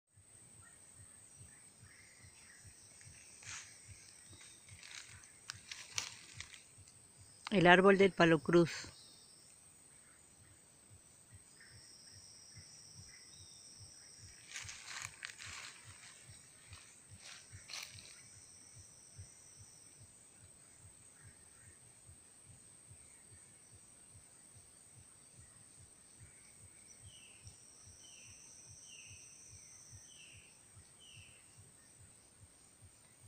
Arbol palo cruz
este arbol al cortarle una pedazo tiene una cruz en el centro.